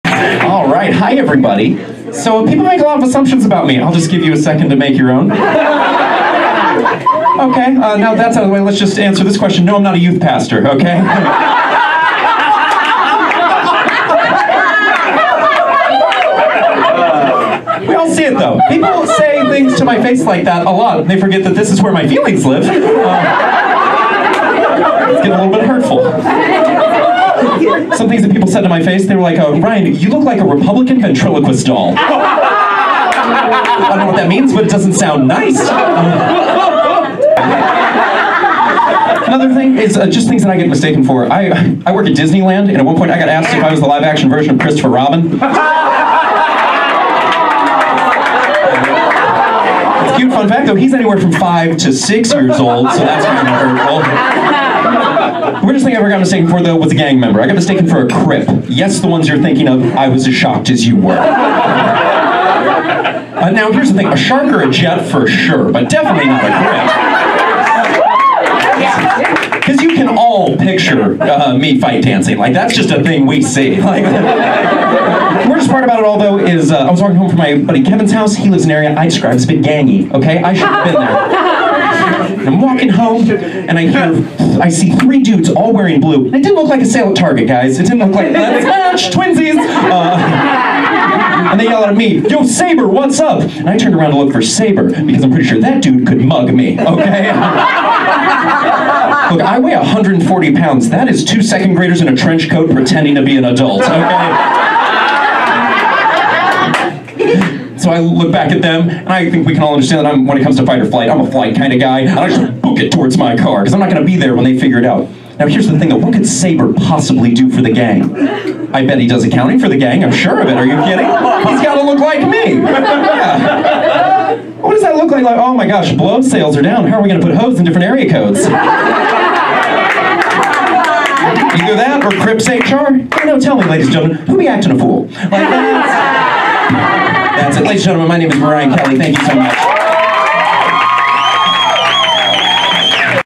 College Comedian: Ryan Kelly: 1-800-993-NEON
Ryan Kelly was born and raised in St. Louis, Missouri where he started comedy as an improviser.